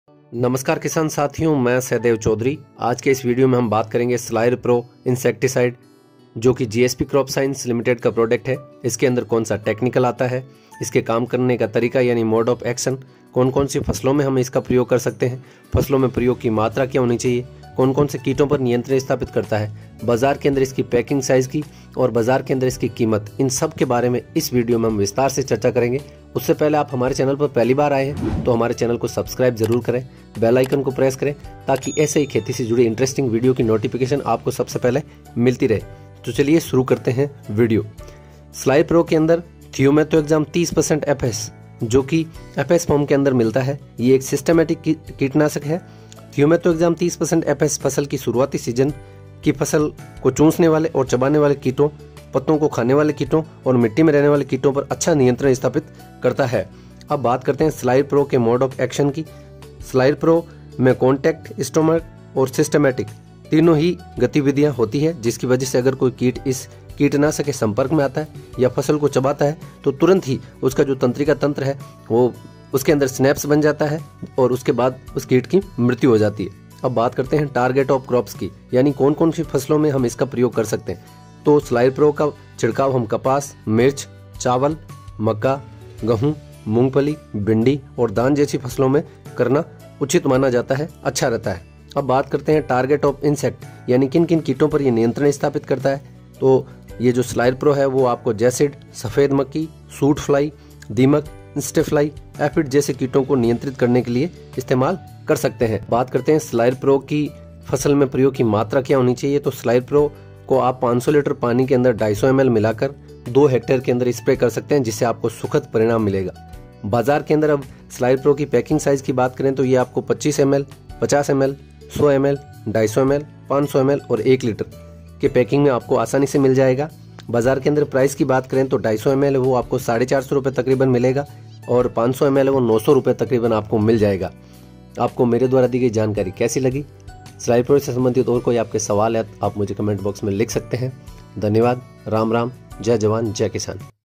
slayer pro insecticide।full detail।GSP। how to work on insect।insecticide slayer pro।
slayer pro के बारे में संपूर्ण जानकारी। मात्रा कीमत काम करने का तरीका । विस्तार से बताया@AgroFarmingIndia @GreenRajasthan21 @anaaj987 @FarmerTalk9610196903 @AgricultureFarmer